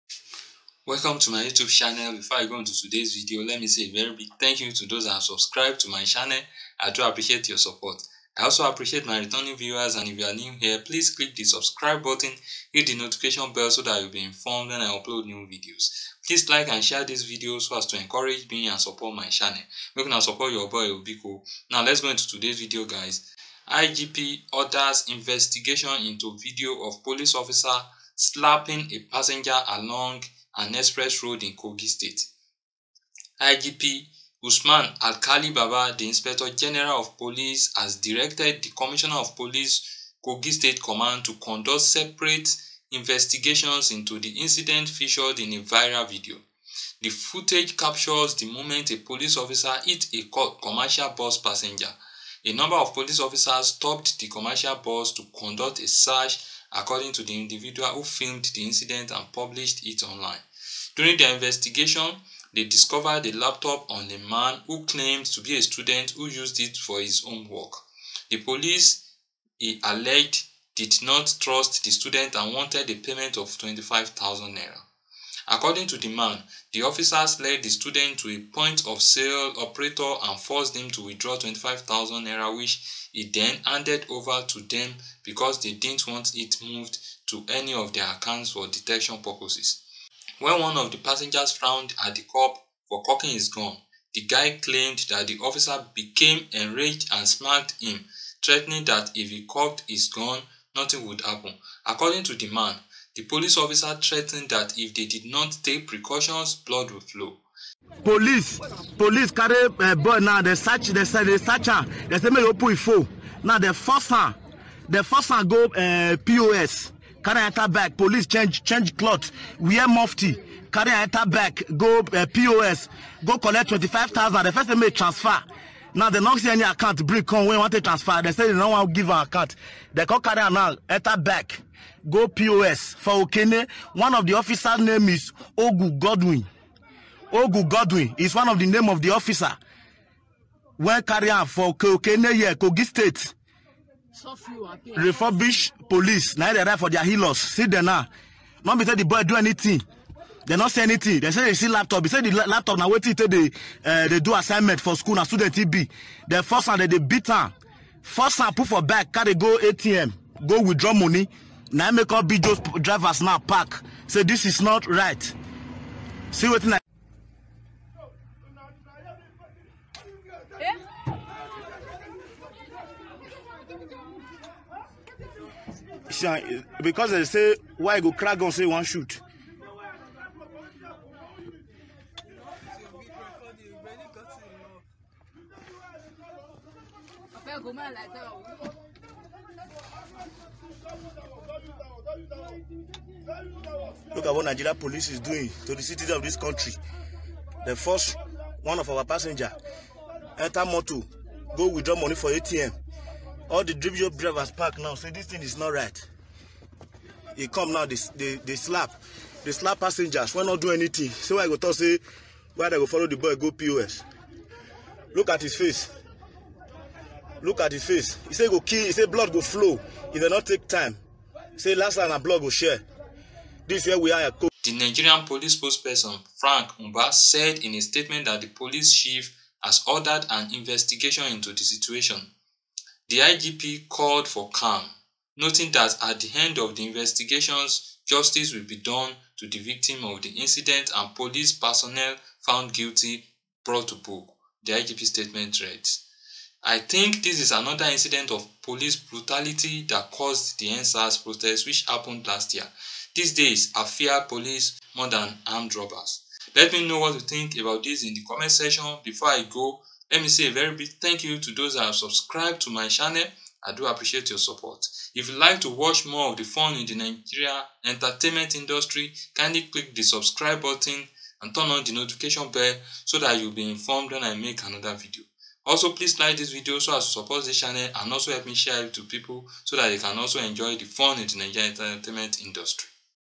CELEBRITY GIST: WATCH VIRAL VIDEO OF POLICE OFFICER SLAPPING A PASSENGER ON EXPRESS IN KOGI STATE
CELEBRITY GIST: WATCH VIRAL VIDEO OF POLICE OFFICER SLAPPING A PASSENGER ALONG AN EXPRESS ROAD IN KOGI STATE